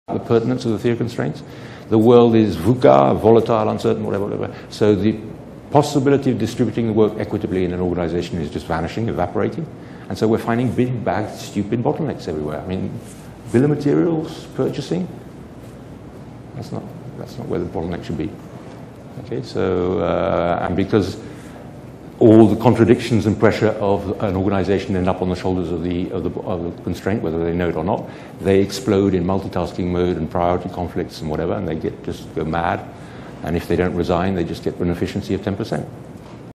The pertinence of TOC in a VUCA World
It is not possible to distribute the work equitably in an organisation any more. So make sure that you do not have your bottleneck in the wrong place.

Apply the 5 Focusing steps of the Theory of Constraints and apply step 5 (repeat) until you get to the bottleneck you wanted. At this point, manage your system according to the Drum Buffer Rope mechanism.

TOC is becoming more and more relevant as the World is becoming more and more VUCA.

Marris Consulting is a management consulting firm.
Our motto: Factories, People & Results.